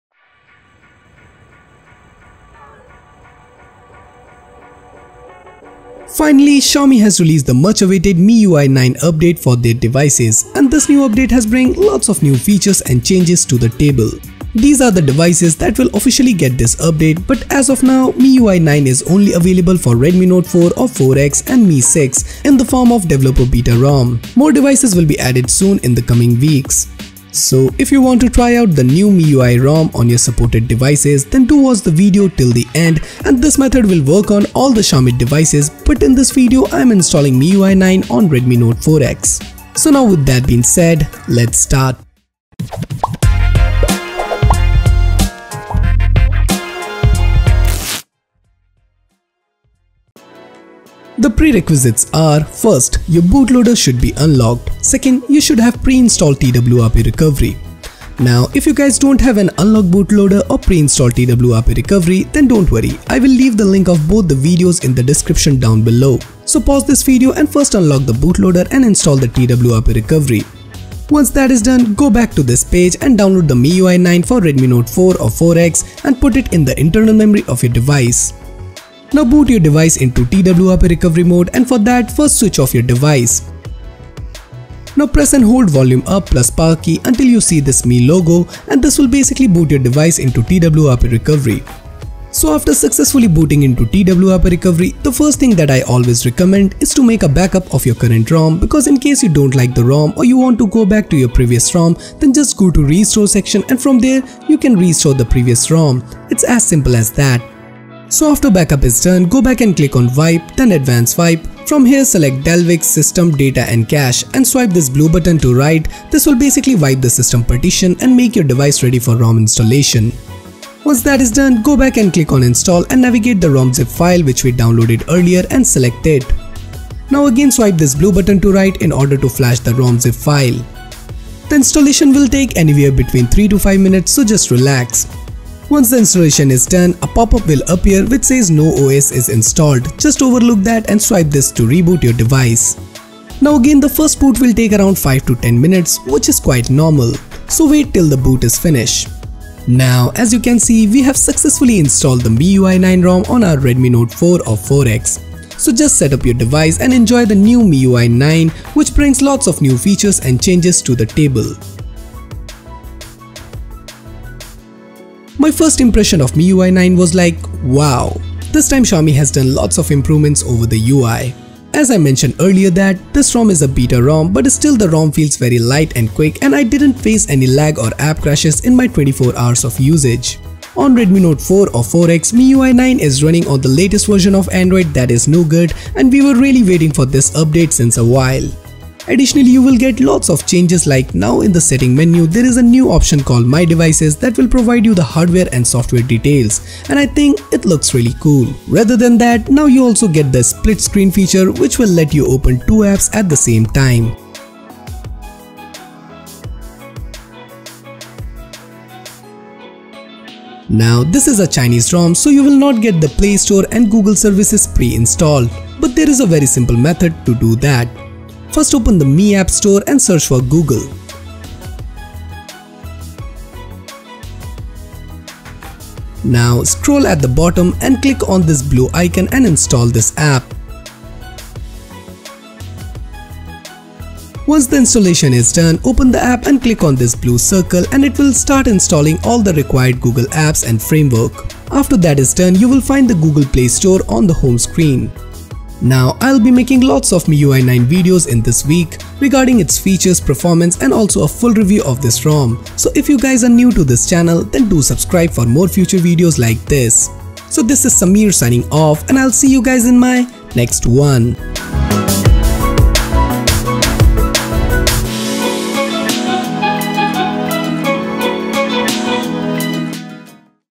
MIUI 9 For Redmi Note 4/4x - How To install (Step By Step Guide)
In this video i am going to show how to install MIUI 9 For Redmi Note 4/4x and how you can get the google apps.

[Video Guide to Unlock Bootloader & Install TWRP recovery]

[Previous Videos that you missed]